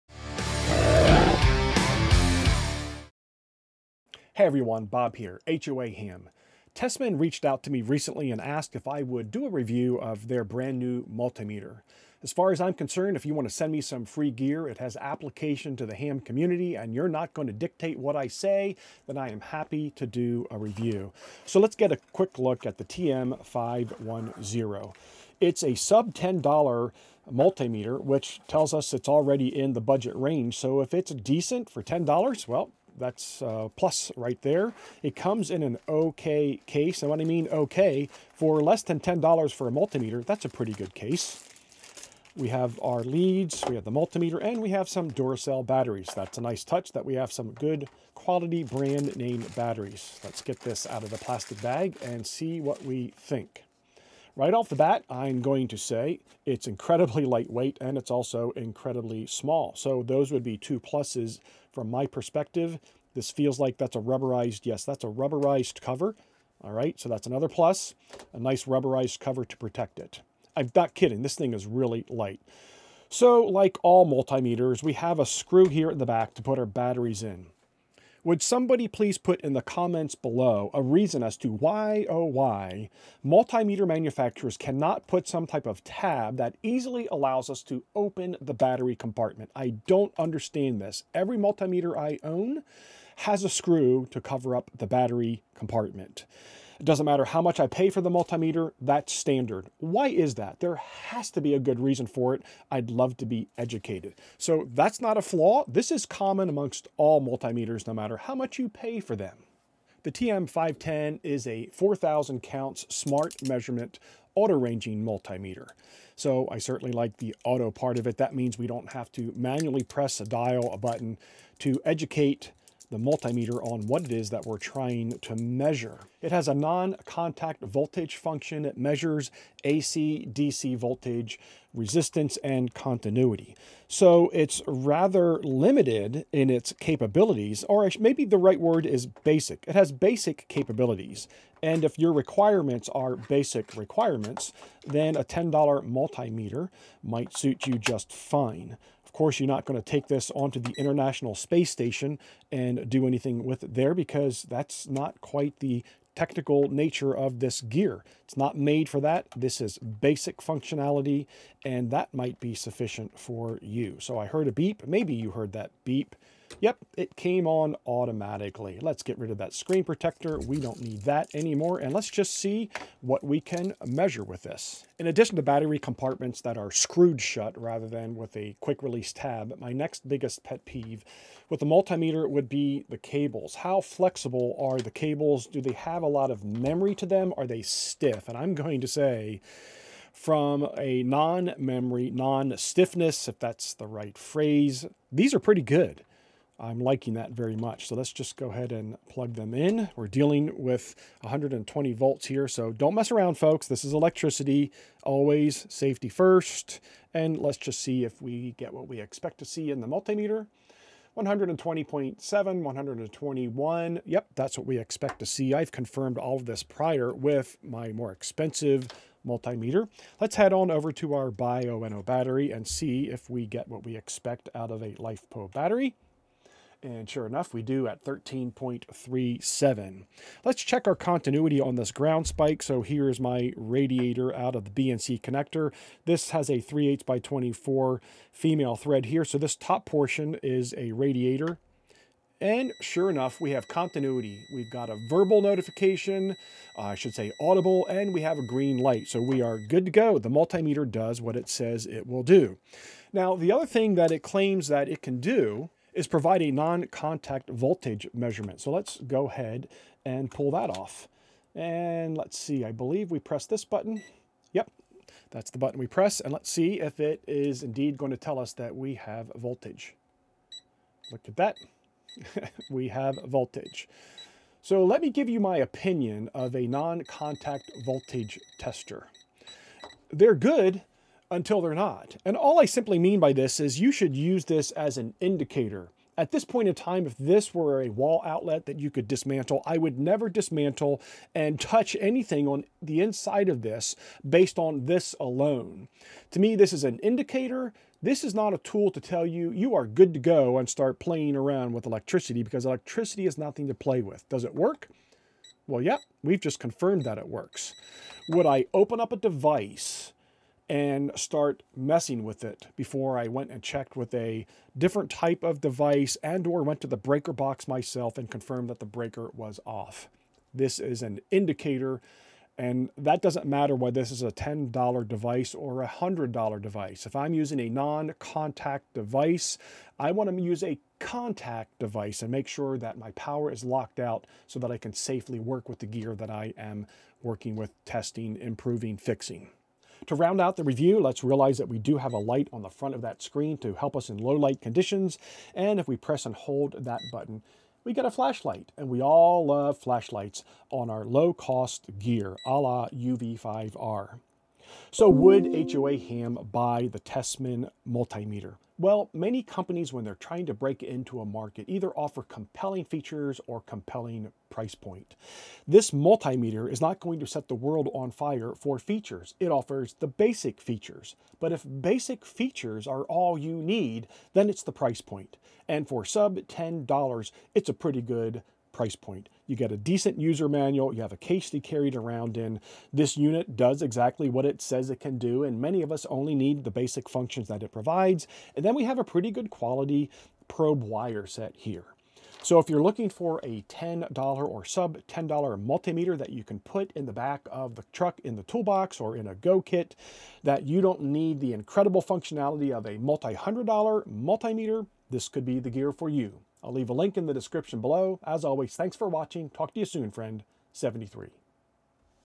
Review of the Tesmen TM-510 Multimeter: Basic Functionality for a Ridiculously Low Price
Review of the Tesmen TM-510 Multimeter.  If all you need is Basic Functionality, you won't  find a nicer unit for this low of a price.

➽ Tesmen Multimeter